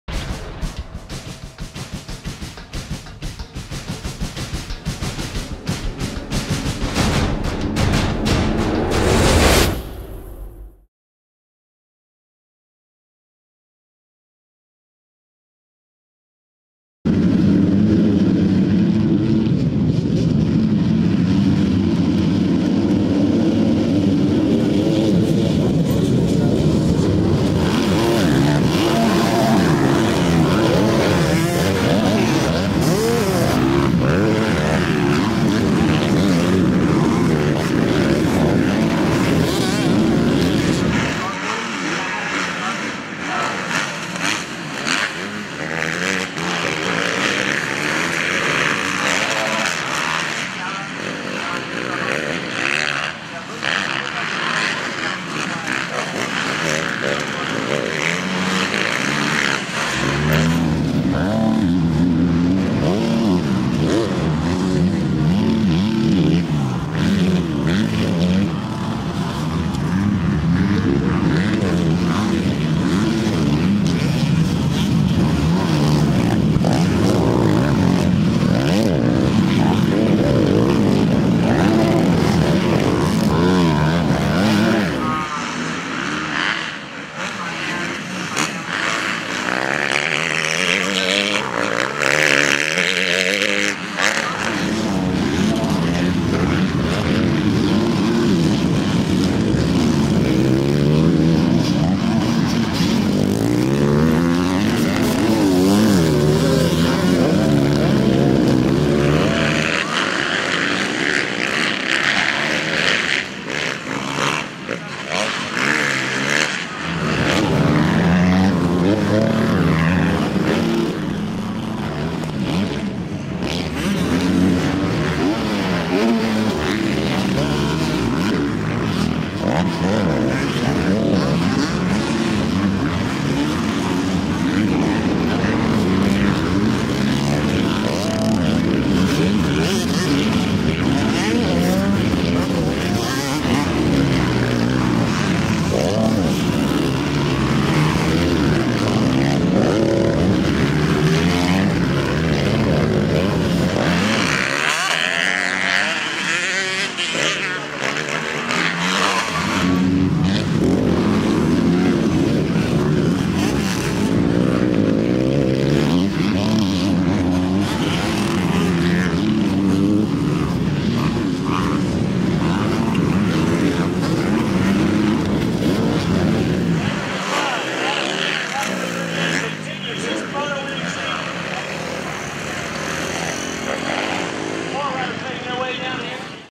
BATTLE: 250 C Modified SX Main Event - Mini O's (MXPTV)
Highlights from the 250 C Modified main event at the 41st annual Winter National Olympics (Mini O's) at the Gatorback Cycle Park near Gainesville, Florida.